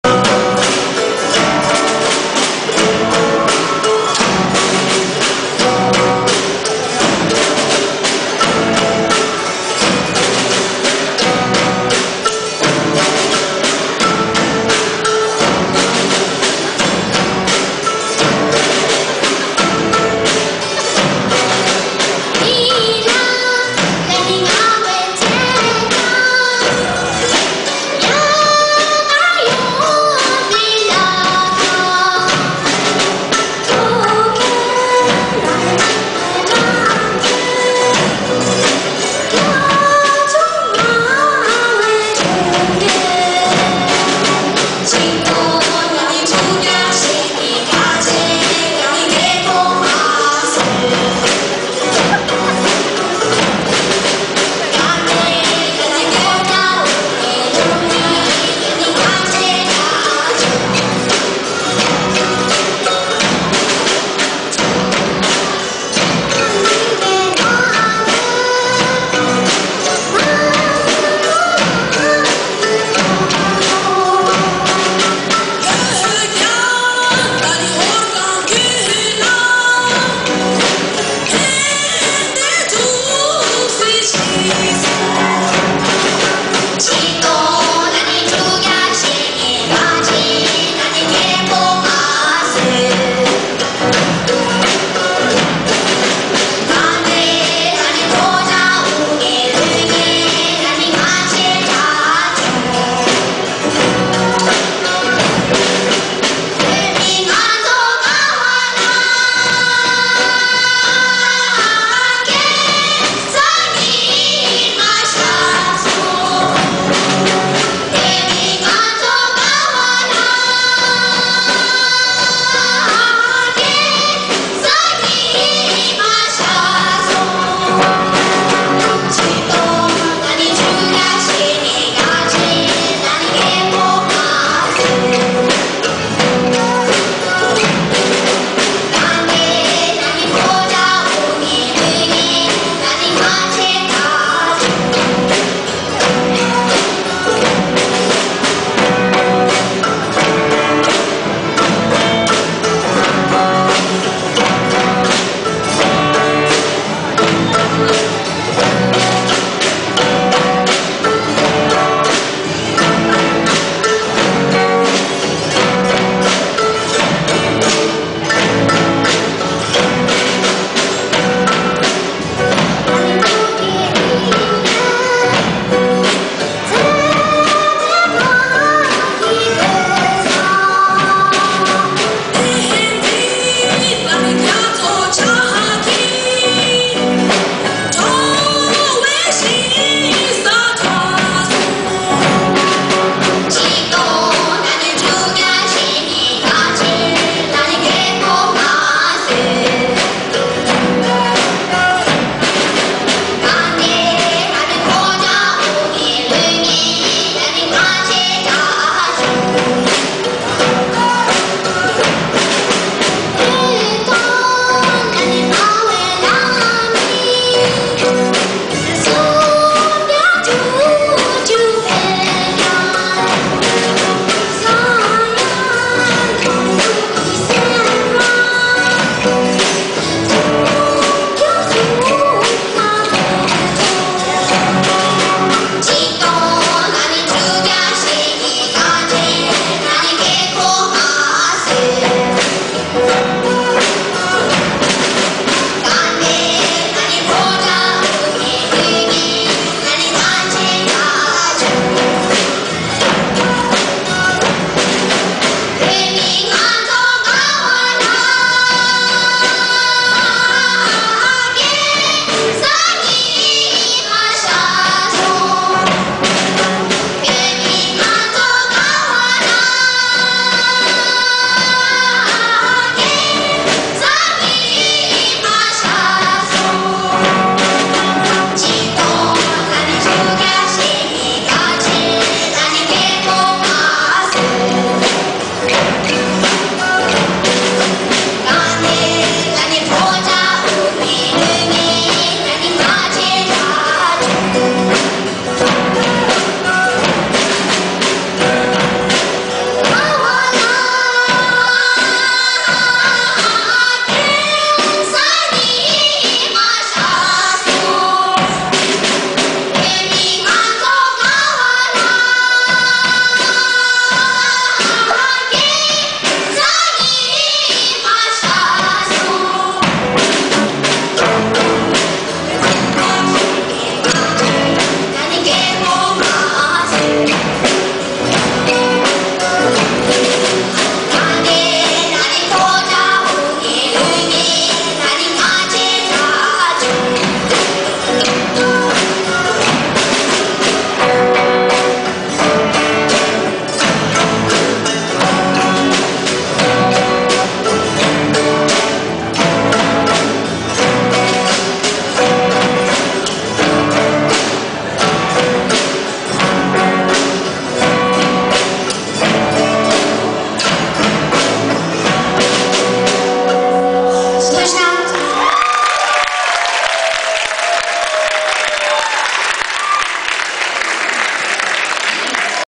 midwest MN 2010 - MN group
Minnesota Cultural Dance group displays amazing singing and instrument skills during Midwest celebration